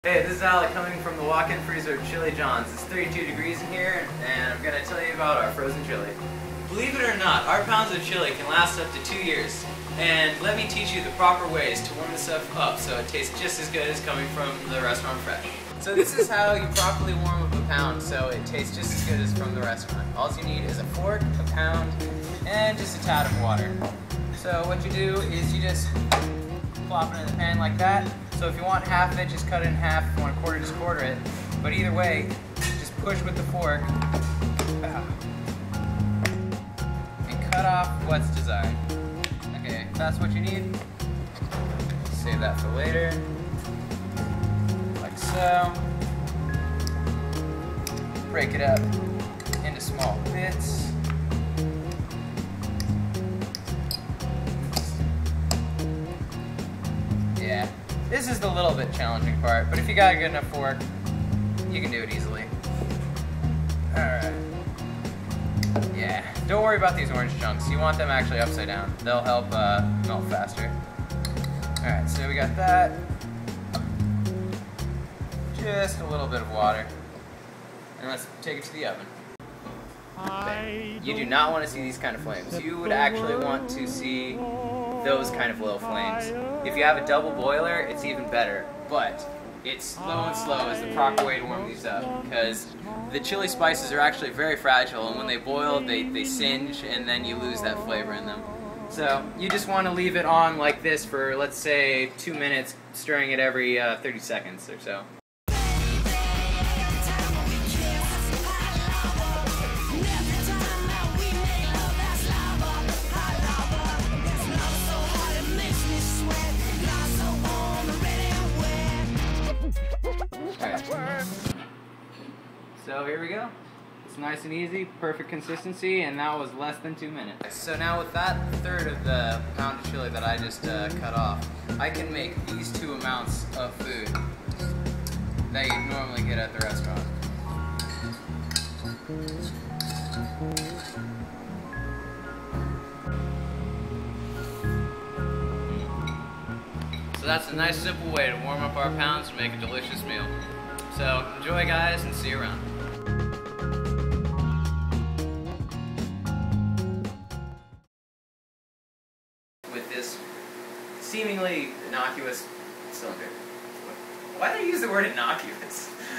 Chili John's: How To Properly Make Frozen Chili John's at Home
[Reheat instructions]

1. Flip pound over and slam into cooking pot.
2. Shave off desired amount with a strong fork and mash into small bits.
3. Add 2 table spoons of water to help the chili melt.
4. Cook on low, stir every 30 seconds till hot (2-4min).
5. Enjoy!